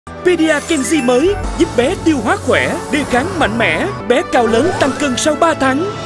PEDIA KENJI – TIÊU HÓA KHỎE, BÉ CAO LỚN, TĂNG CÂN SAU 3 THÁNG
Pedia Kenji Mới với các dưỡng chất chuyên biệt giúp bé tiêu hóa khỏe, đề kháng mạnh mẽ, cao lớn, tăng cân sau 3 tháng
+ Chất xơ GOS từ Nhật Bản hỗ trợ tiêu hóa khỏe mạnh, hạn chế táo bón.
+ Hàng tỷ lợi khuẩn BB12 & LGG
+ Canxi Nhật và Vitamin K2 giúp phát triển chiều cao vượt trội
Sản phẩm đã được chứng minh lâm sàng trên trẻ em Việt Nam*:
+ Tăng cân, tăng chiều cao sau 3 tháng
+ Giảm 23% tỉ lệ rối loạn tiêu hóa
+ Giảm 50% tỉ lệ sốt ho.

*Theo báo cáo hiệu quả bổ sung sản phẩm dinh dưỡng Pedia Kenji 2+ lên tình trạng dinh dưỡng, biếng ăn, rối loạn tiêu hóa, nhiễm khuẩn đường hô hấp ở trẻ 24-59 tháng tuổi năm 2020